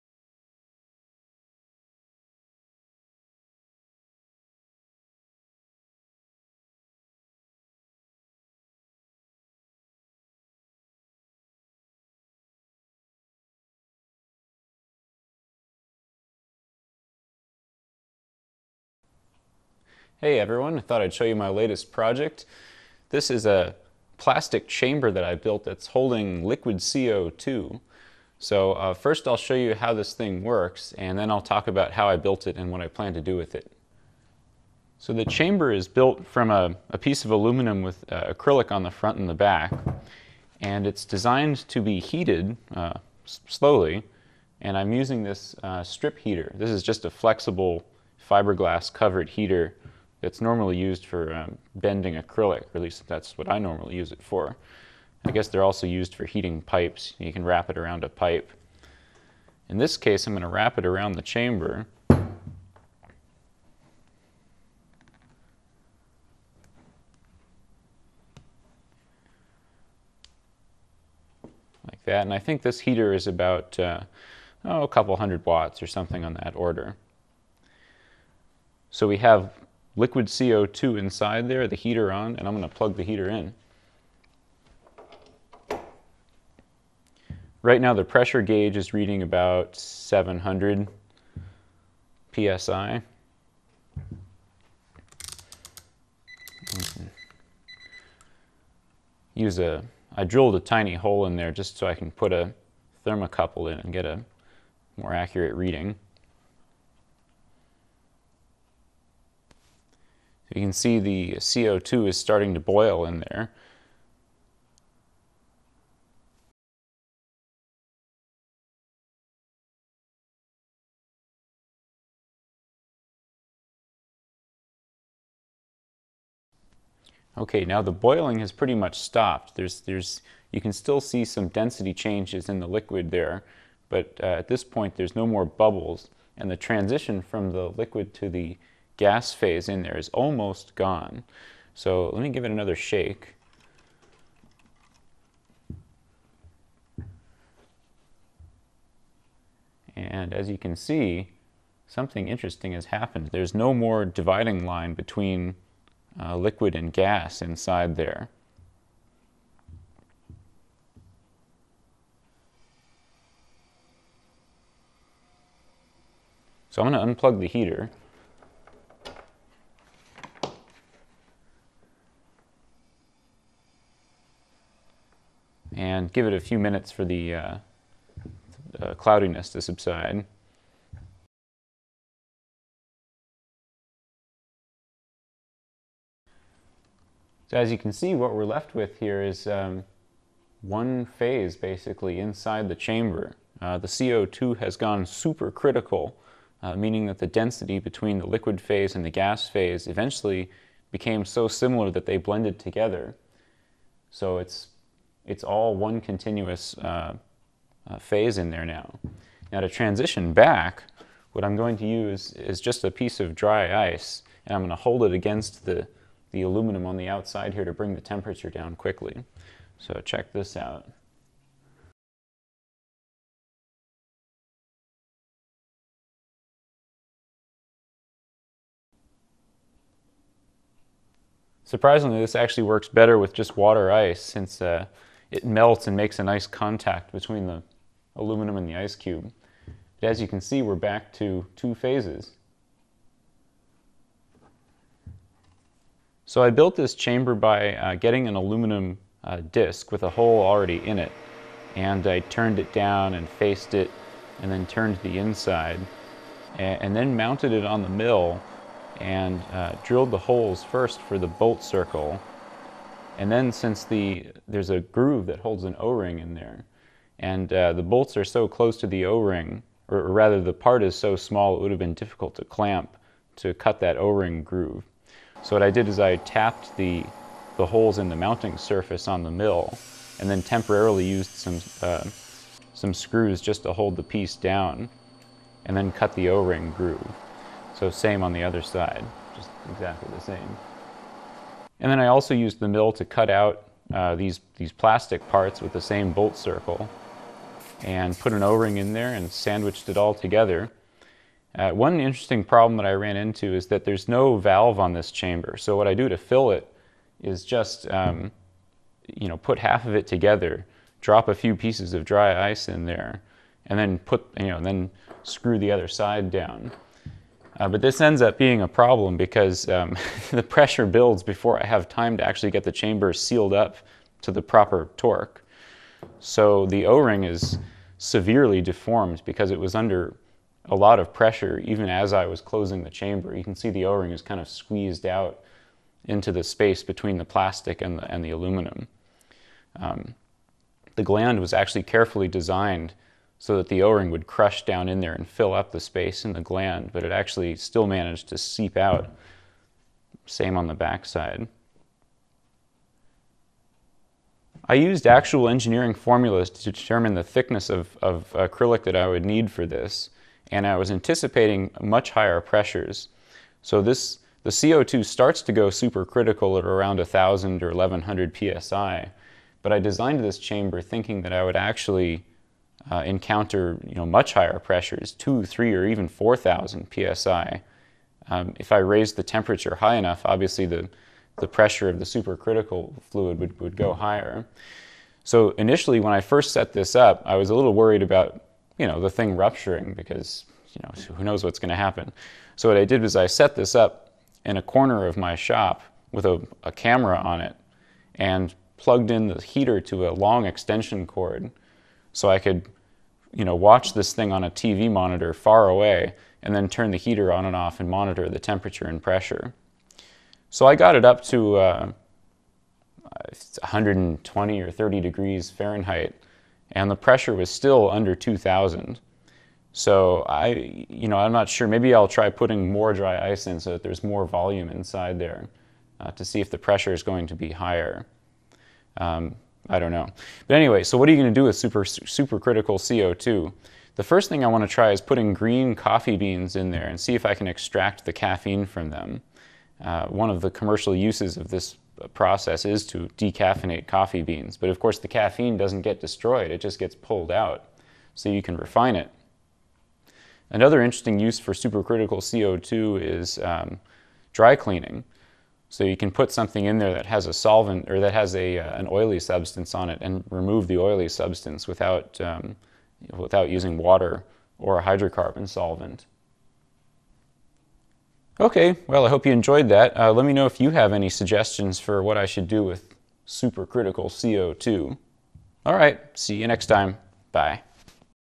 A close look at supercritical carbon dioxide CO2
I built a pressure vessel from aluminum and acrylic, and filled it by placing pieces of dry ice inside.  The dry ice melts under high pressure, and forms a liquid and gas phase.  When the vessel is heated, the CO2 becomes supercritical -- meaning the liquid and gas phases merge together into a new phase that has properties of a gas, but the density of a liquid.

Supercritical CO2 is a good solvent, and is used for decaffeinating coffee, dry cleaning clothes, and other situations where avoiding a hydrocarbon solvent is desirable for environmental or health reasons.

It may be important to open the container before all of the solid melts.  When there is still some solid CO2 present, the pressure will be close to the triple point.  Once the solid completely melts, the pressure will increase quickly to about 750 psi depending on the ambient temp.  I really doubt those plastic containers could hold 750 psi.